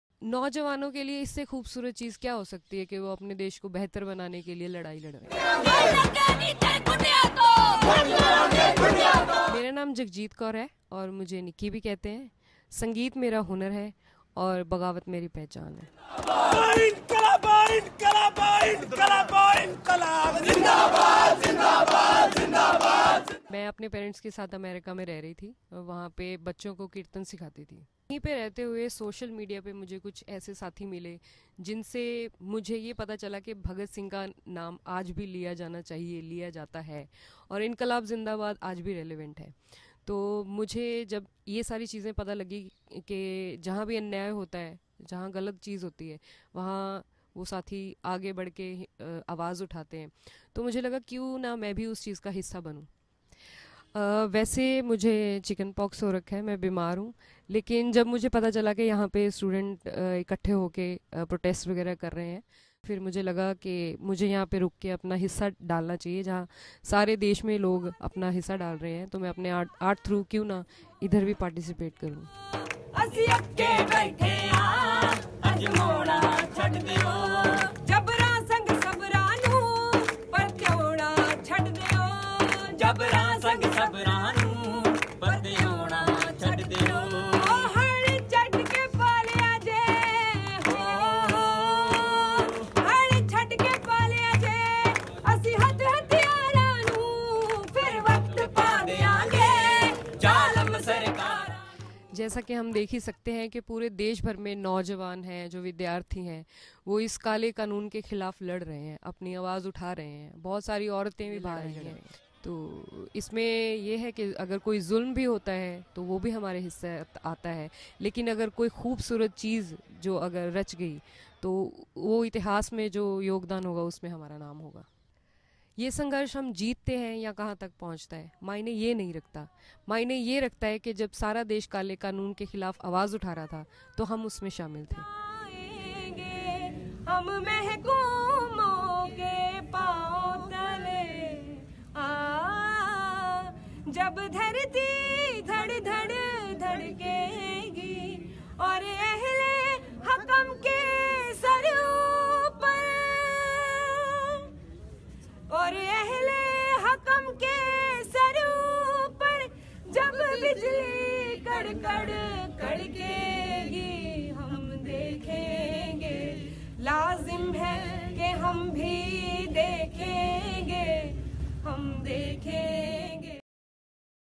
India Protests: Jagjeet Kaur, who sing Songs of Protest- BBCURDU
انڈیا کی جگجیت کور موسیقی کی ریسرچ سکالر ہیں اور احتجاجی نغمے گاتی ہیں۔
وہ سنہ 2012 میں امریکہ چھوڑ کر انڈیا آئیں اور 2014 میں سٹوڈنٹ ایکٹوسٹ کے طور پر کام کرنا شروع کیا۔
انڈیا میں شہریت کے متنازع بل پر جاری طلبا کے احتجاج پر جگجیت کہتی ہیں کہ نوجوانوں کے لیے اس سے خوبصورت چیز کیا ہو سکتی ہے کہ وہ اپنے ملک کو بہتر بنانے کے لیے لڑائی لڑیں۔